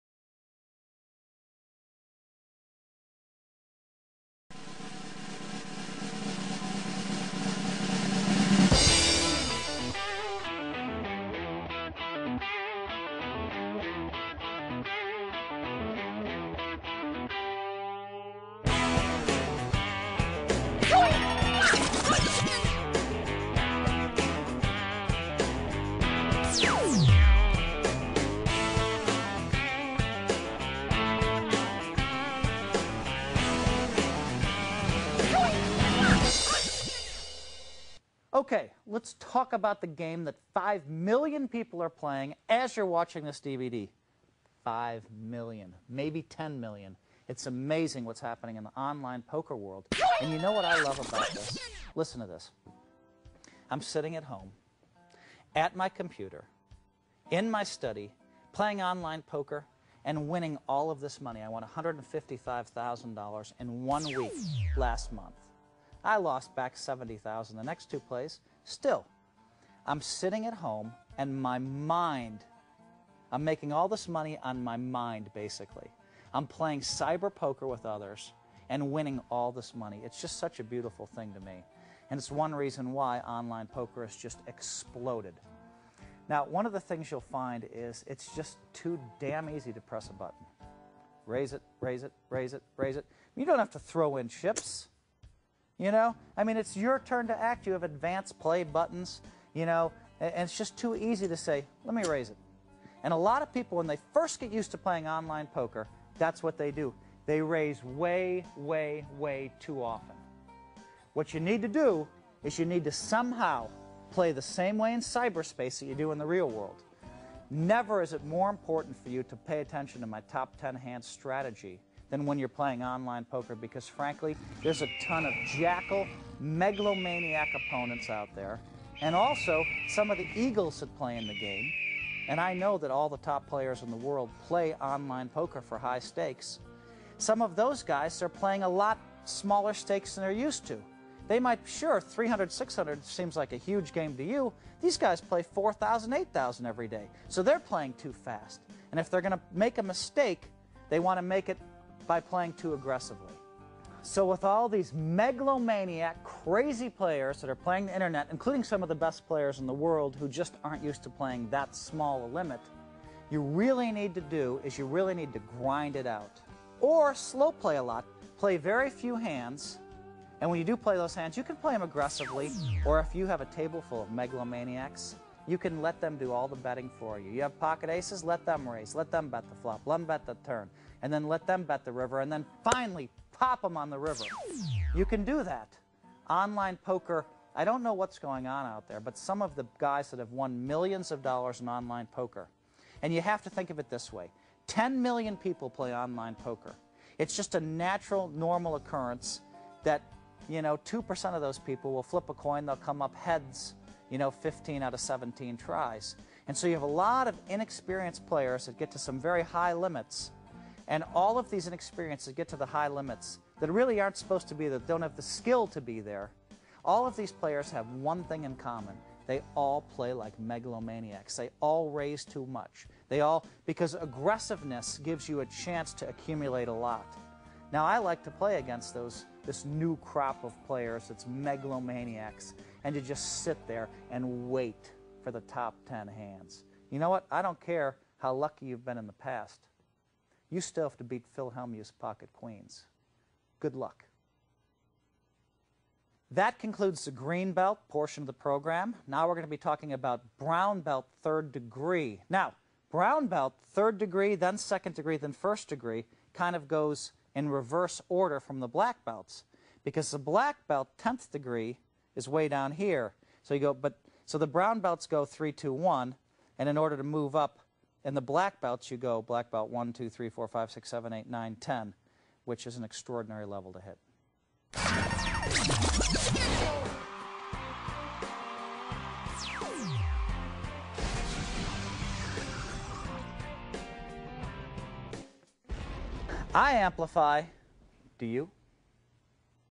Phil Hellmuth: Online Poker Secrets
In this video, I talk about stepping up your online poker game.